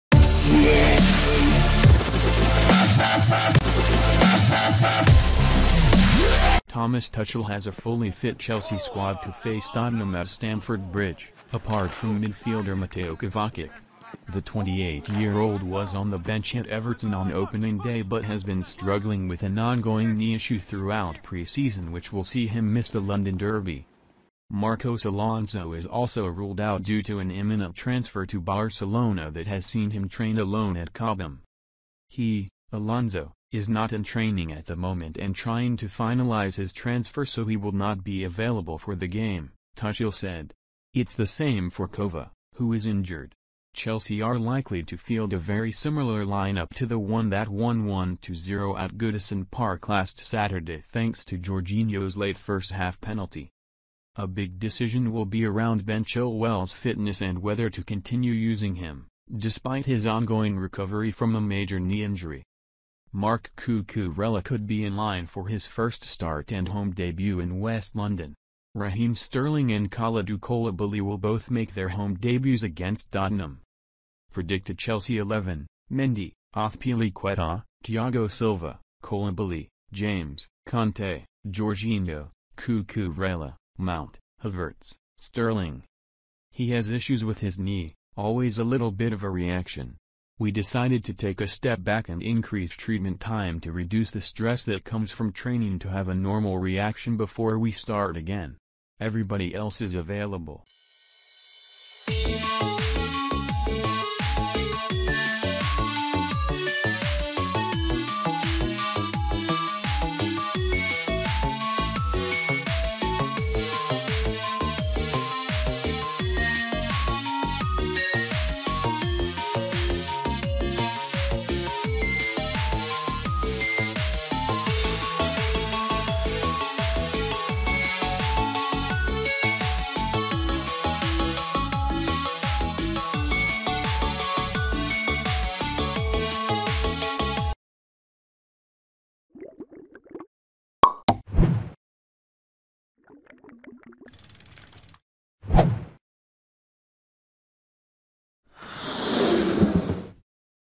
Chelsea XI vs Tottenham Predicted lineup Marc Cucurella and Raheem Sterling start
Thomas Tuchel has a fully-fit Chelsea squad to face Tottenham at Stamford Bridge, apart from midfielder Mateo Kovacic.

The 28-year-old was on the bench at Everton on opening day but has been struggling with an ongoing knee issue throughout pre-season which will see him miss the London derby.

Marcos Alonso is also ruled out due to an imminent transfer to Barcelona that has seen him train alone at Cobham.